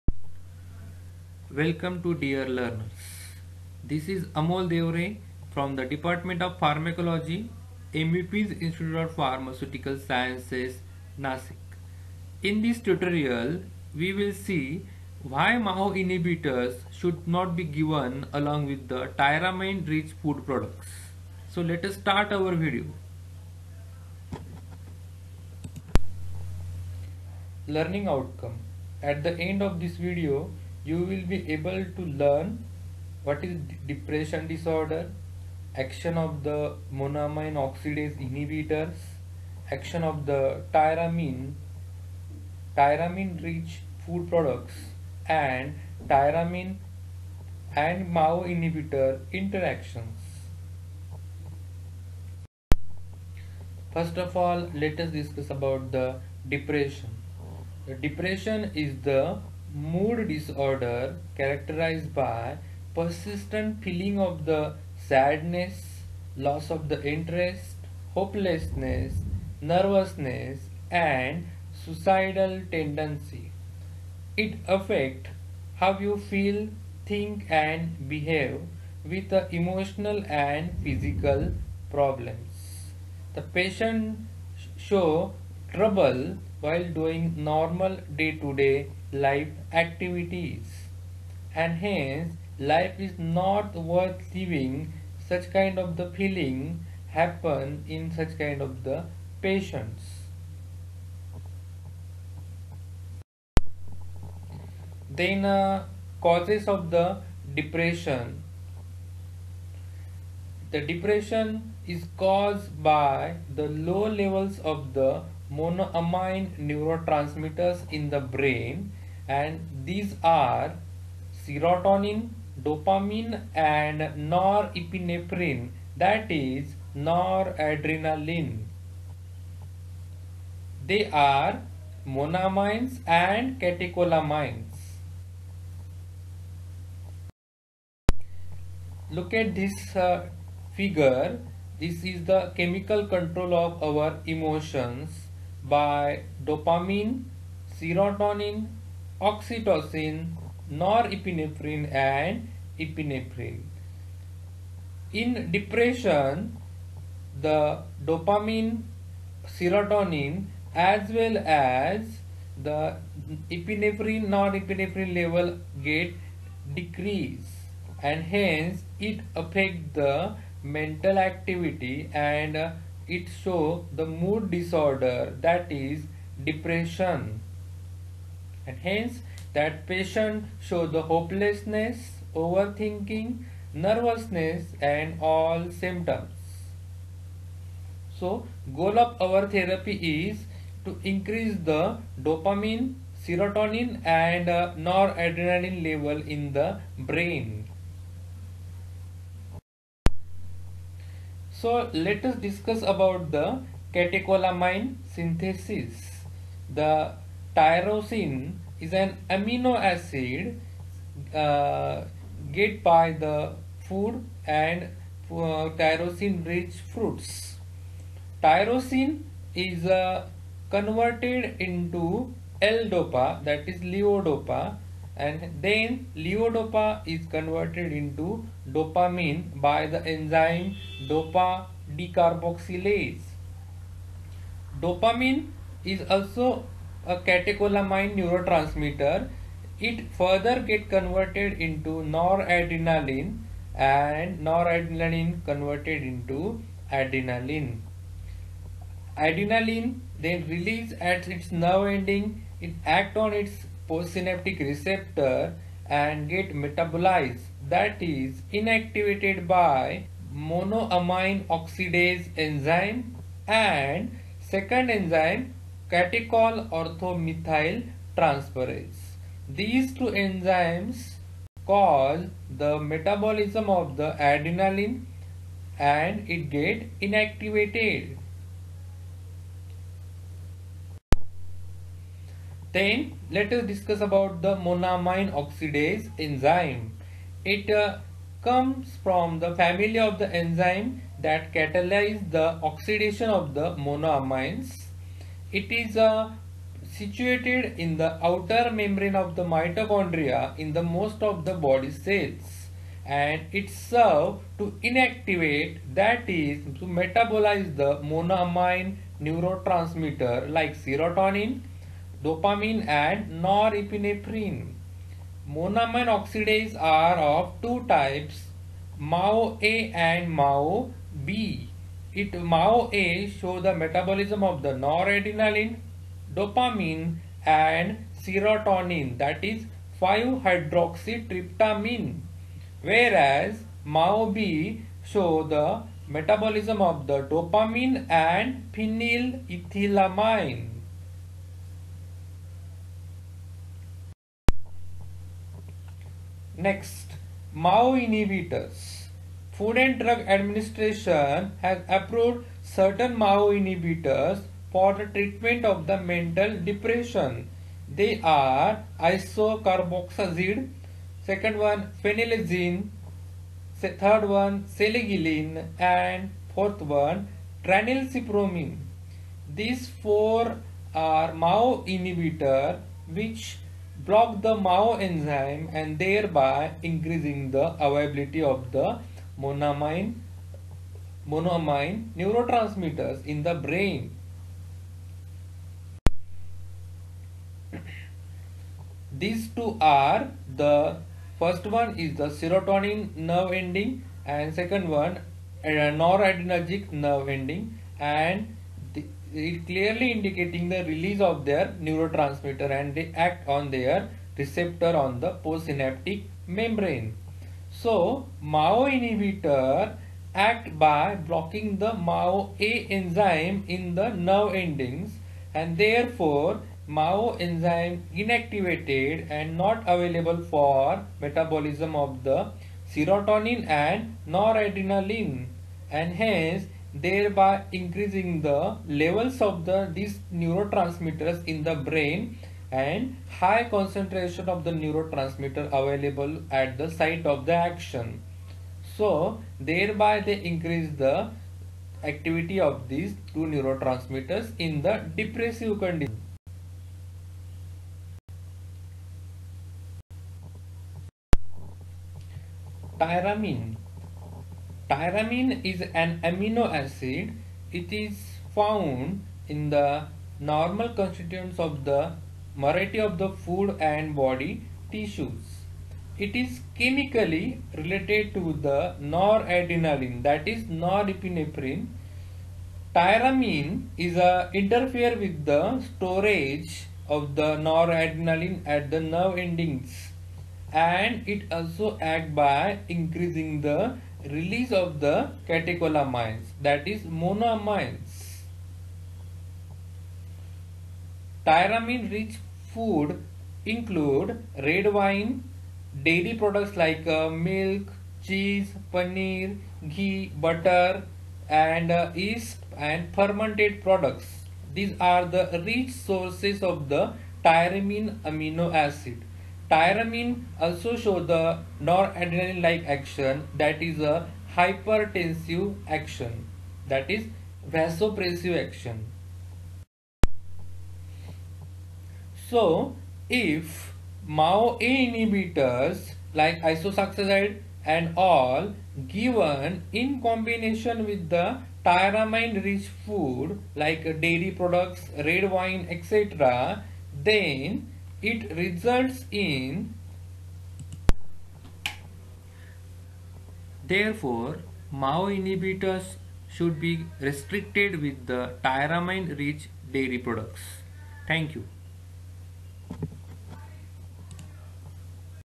MAO inhibitors contraindicated with Tyramine rich food
Tyramine is an amino acid which is utilized for synthesis of catecholamines. Catecholamines help regulate blood pressure.
 Tyramine present naturally in the body and also found in cheese, chocolates, citrus fruits, chicken liver, yeast, dairy milk products, red wines etc.
 Monoamine oxidase enzyme promote metabolism of tyramine in the body whereas Monoamine oxidase inhibitors (MAOIs) block monoamine oxidase enzyme.
 Blocking this enzyme helps relieve depression by raising the levels of catecholamines (NA, adrenaline, dopamine) and serotonin in the brain.
 If MAO inhibitors and high-tyramine rich food taken simultaneously then tyramine can quickly reach dangerous levels. This can cause a serious hypertensive crisis (Alpha and Beta adrenergic action) and require emergency treatment.
 Hypertensive crisis include severe hypertension, cardiac arrhythmia, cerebral hemorrhage etc.
 Therefore avoid taking MAO inhibitor therapy with consumption of dairy products as well as red meat.